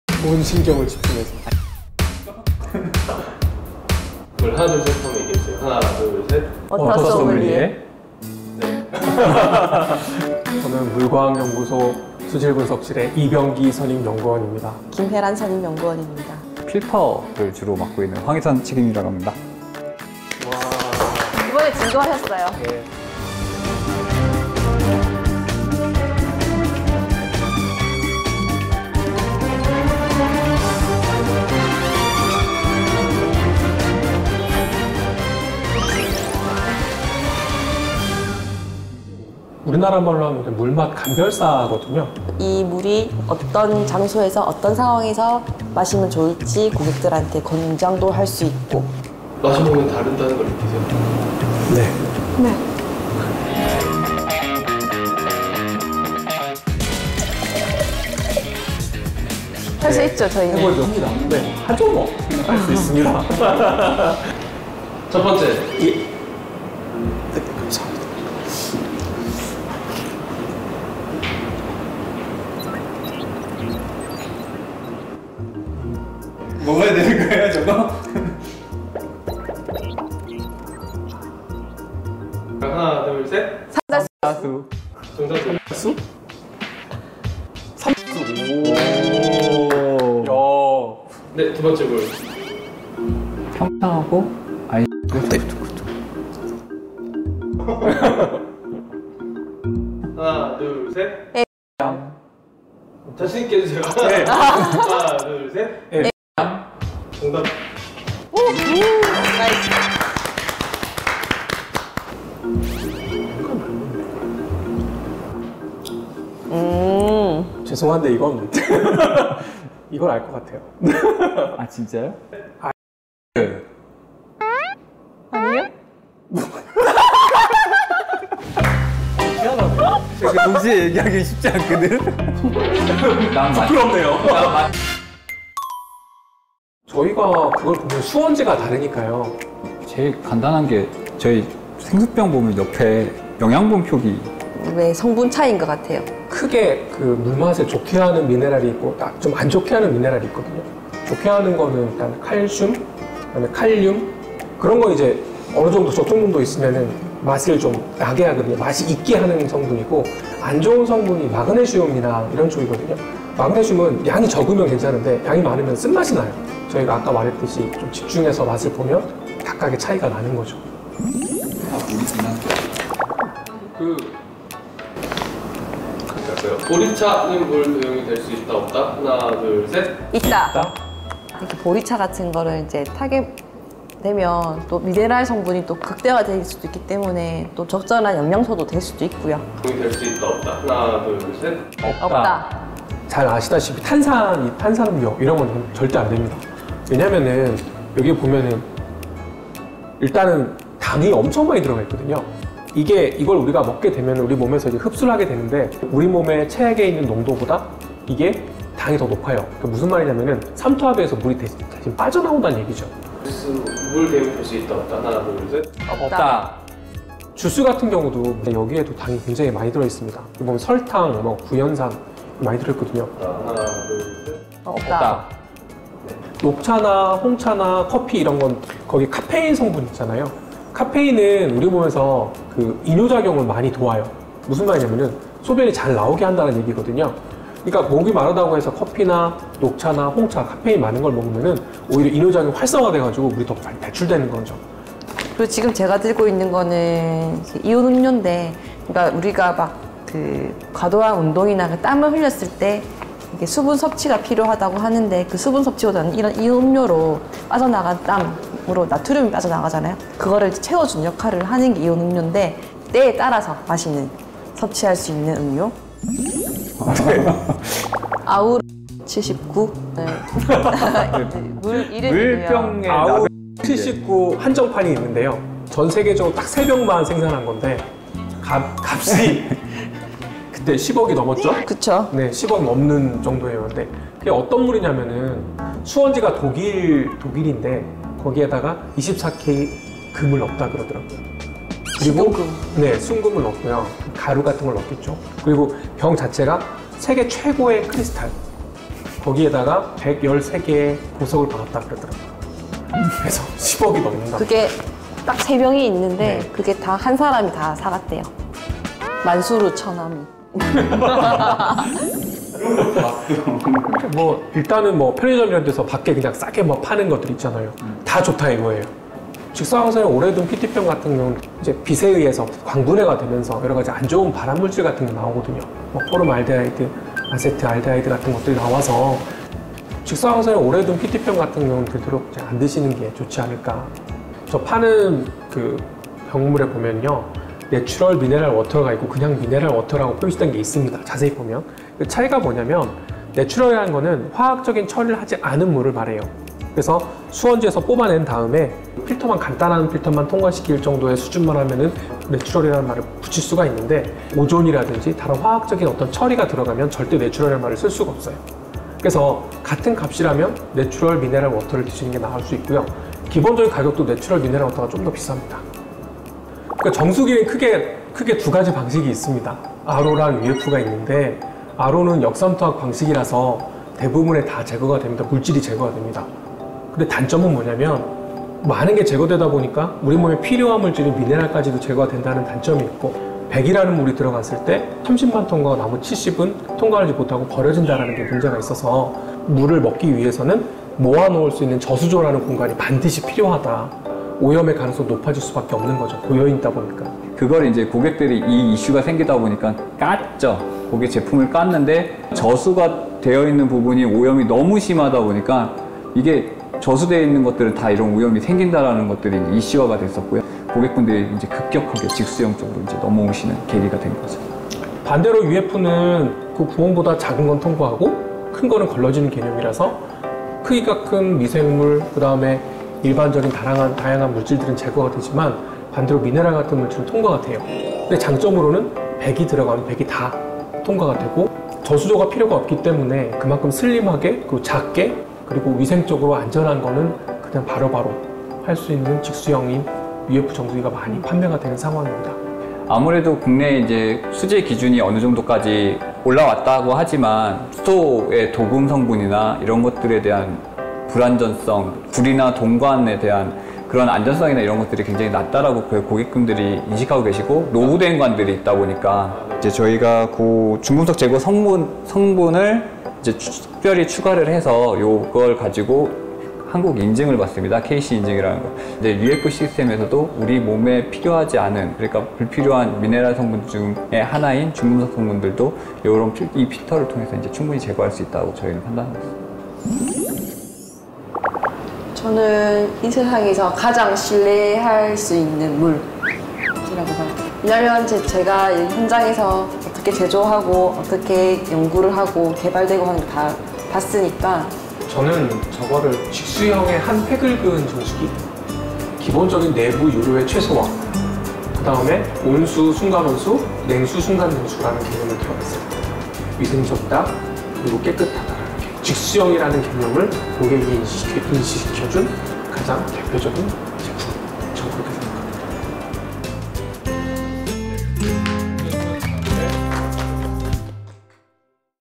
[리얼리뷰] 물 전문가가 소개하는 LG 퓨리케어 정수기
물 맛까지 구분해내는 LG전자 물과학 연구소 연구원들이 알려주는
‘물’에 대한 알찬 정보! 💧